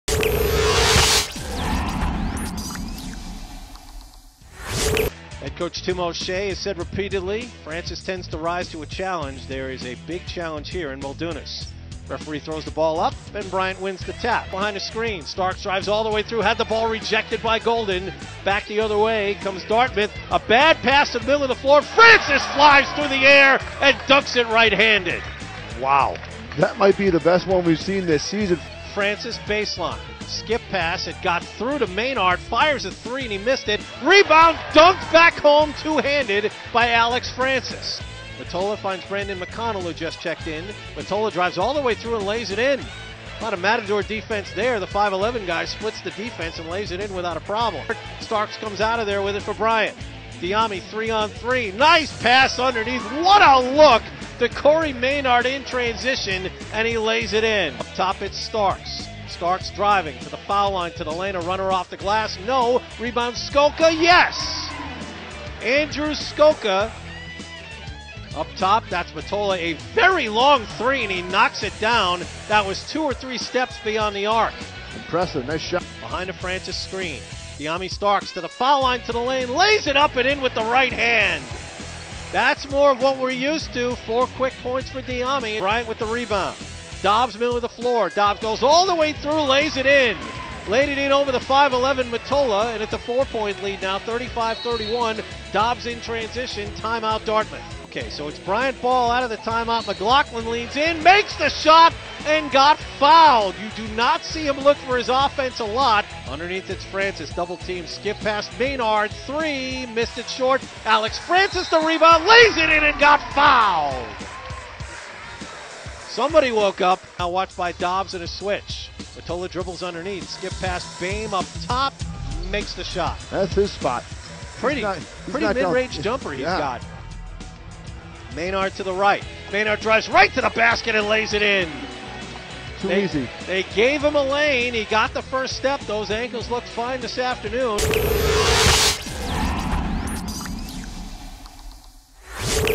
Men's Basketball Highlights vs. Dartmouth
Bryant improved to 6-4 with a 79-66 win over Dartmouth Saturday afternoon.